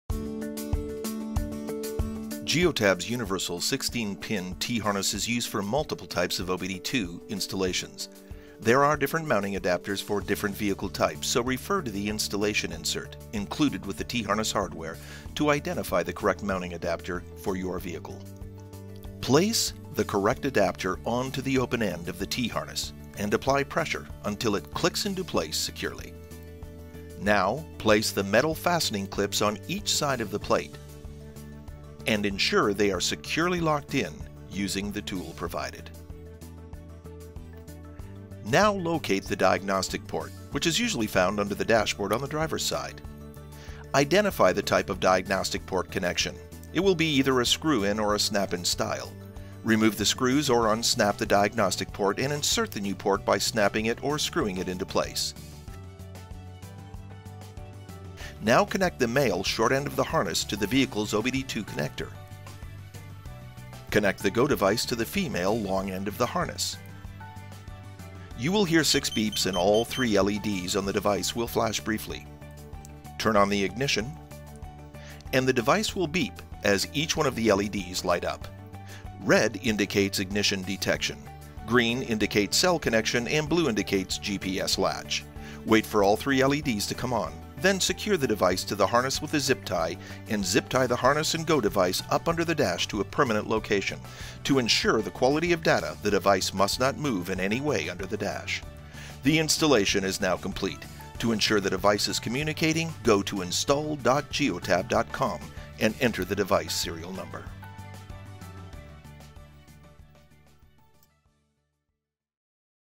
How to Install Geotab's Universal OBD II T-Harness Kit | Vehicle Tracking Device Installation
Learn how to install your Geotab Vehicle Tracking Device in any light or medium-duty vehicles using our Universal OBD II T-Harness Kit.

To ensure the device is communicating visit install.geotab.com after completing installation.

Read more about installing the Universal 16-Pin T-Harness Kit at the Geotab Blog

Learn more about the fleet management solutions from Geotab and find an authorized Geotab Reseller

Installers verify your installations at install.geotab.com